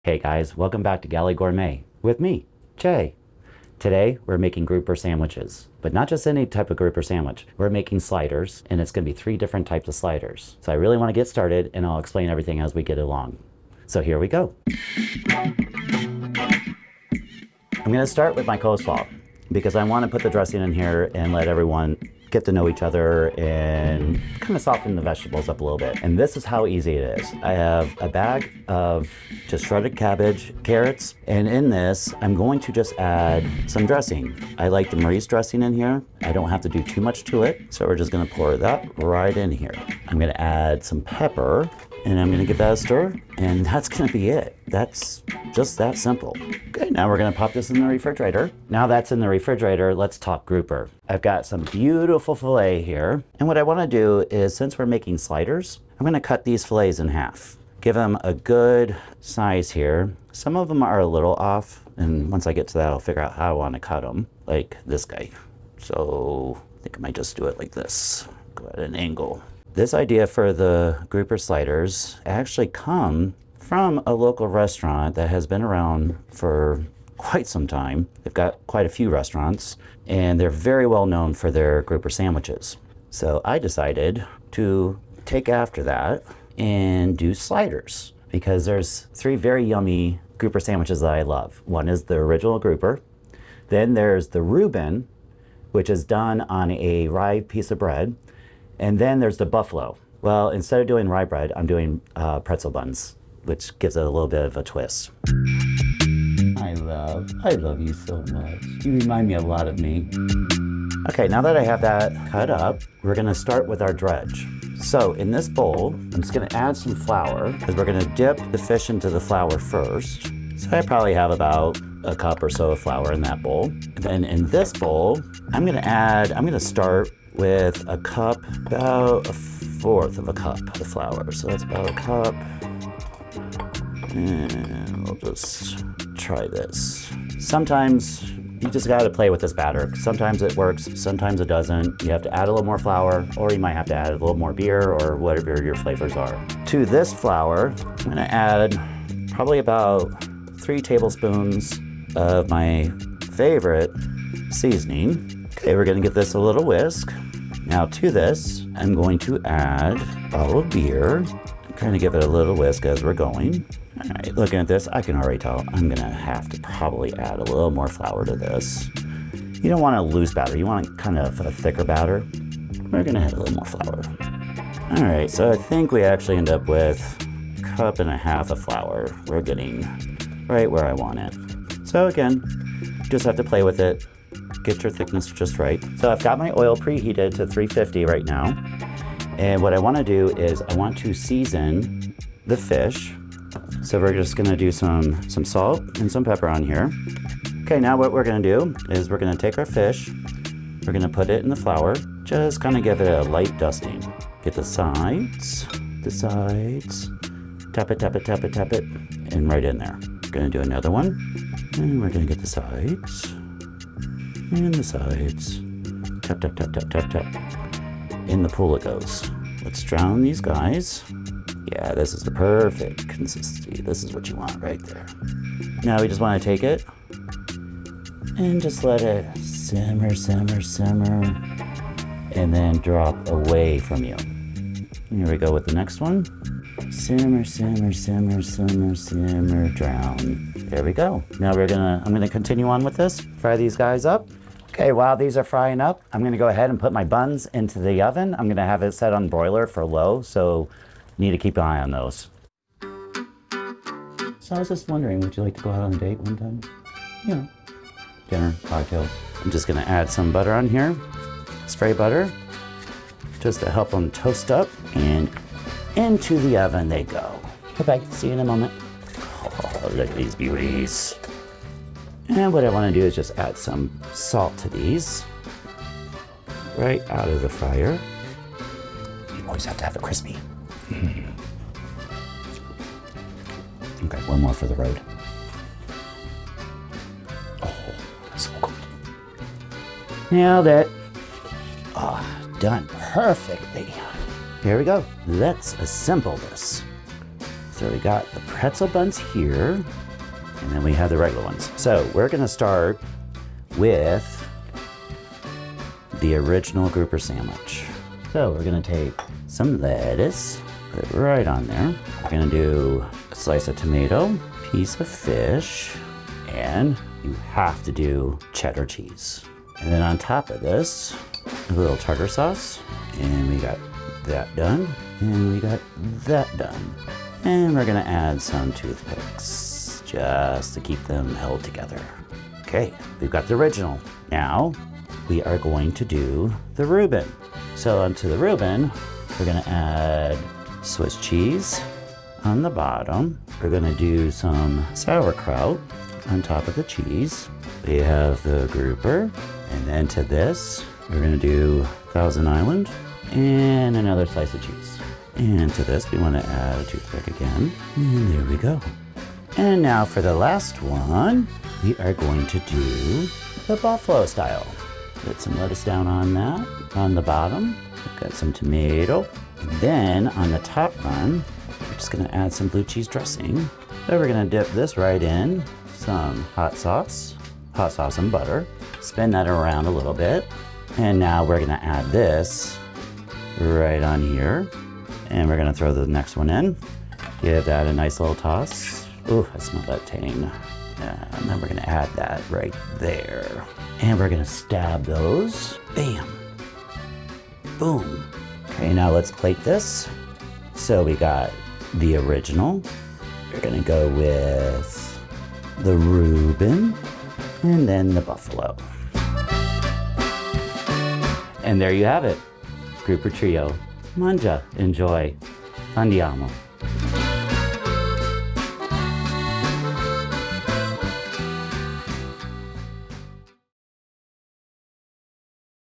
Three Delicious Fish Sliders | Elevate Your End-of-Summer BBQ
Hamburgers and hot dogs are classic BBQ staples 🍔🌭 but let’s mix things up with three new slider options that will impress your guests without doubling the effort. We’re cooking one big batch of fish to create the sliders: the Original, the Reuben, and the Buffalo. There’s something for everyone! 🐟🍽️

These sliders are inspired by some of my favorite local restaurants in Tampa, Florida, where the grouper sandwiches are so incredible that I couldn’t decide which one to order. My solution? Sliders! Mini versions of the sandwiches I loved so I could enjoy them all.

We’re also making the easiest coleslaw ever. Just grab a bag of shredded coleslaw mix, add Marie’s Coleslaw Dressing, a little pepper, and stir. Pop it in the fridge until you’re ready to eat.

1. PREPARE THE FISH

I used grouper for these sliders.
Cut the filets in half.

In a bowl, mix 1 1/2 cups of flour with 3 tablespoons of Chesapeake Bay seasoning (one of my favorites!) Add one beer while whisking. The batter should be thick, not runny — add more flour if needed.

In a second bowl, place at least 1/4 cup of flour.

Preheat oil to 350°F.

Season fish with salt and pepper.

Get ready to dredge your fish!
Start by putting your fish in the bowl of dry flour.
Then put it in the batter mix.
Make sure each piece is covered.

BE CAREFUL WHEN PUTTING YOUR FISH IN THE HOT OIL!
I start by dipping just a small section in, then slowly drop it — AWAY FROM YOU!

Once your fish pieces are fried and removed from the oil, add a little bit of salt.
You want them to be nice and crispy.

2. TOAST THE BUNS

I used regular buns for two of the sliders and pretzel buns for the Reuben.

Set your broiler to low.

Add butter or spray butter to the buns.

Broil and toasted, keeping a close eye on them!

3. ASSEMBLE THE SLIDERS!

The Original:
Bun
Lettuce
Tomato
Fried fish
Cheddar cheese
Tartar sauce
Use toothpicks to secure

The Reuben:
Pretzel bun
Swiss cheese
Bubbie's sauerkraut
Fried fish
Thousand Island dressing
More Swiss cheese
Secure with toothpicks

The Buffalo:
Bun
Lettuce
Tomato
Dip your fried fish in a pan of heated hot sauce and butter until coated
Add to your slider
Blue cheese dressing
Secure with toothpicks

I hope you enjoy these sliders as much as I do!
COMMENT to tell me which one is your favorite.

Andiamo!